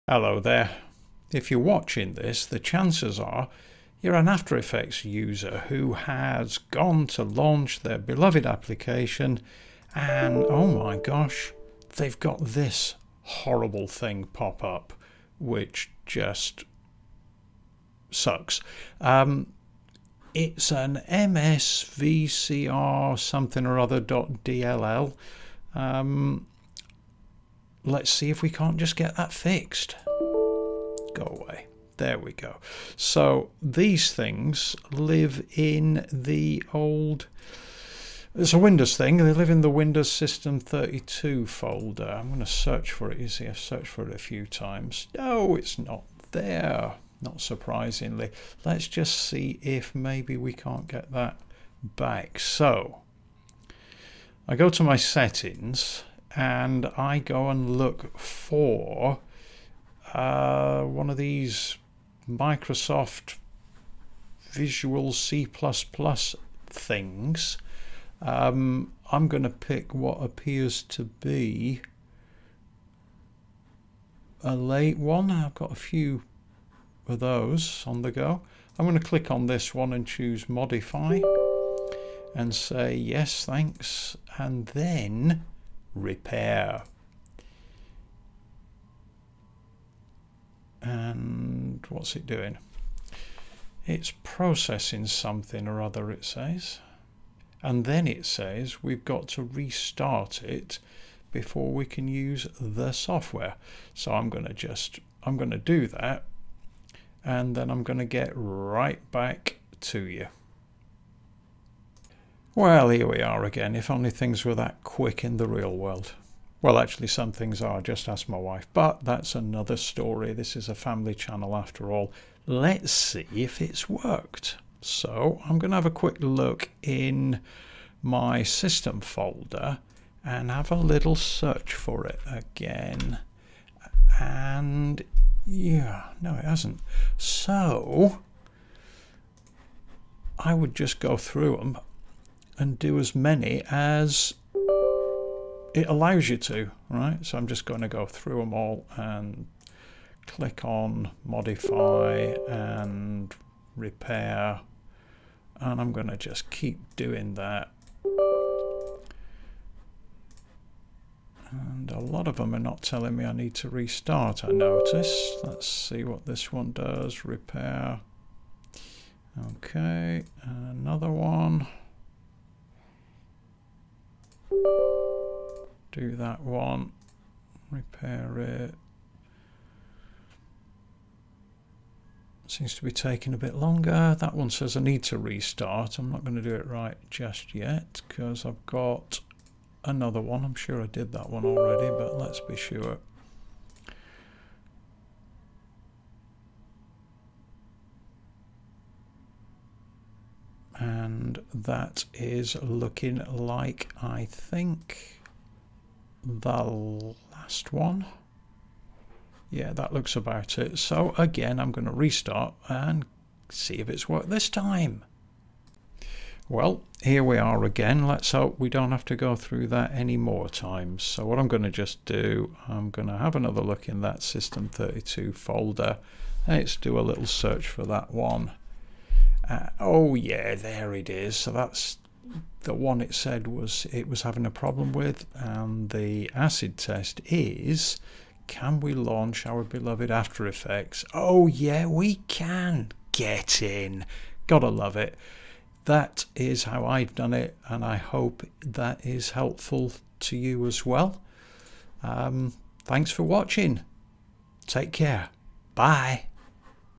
SIMPLE FIX for AFTER EFFECTS MSVCR110.dll PROBLEM NO DOWNLOADS REQUIRED
If your Adobe After Effects will not launch and you get the following message: "The code execution cannot proceed because MSVCR110.dll was not found. Reinstalling the program may fix this problem" then this short and simple tutorial is for you. NO DOWNLOADS REQUIRED.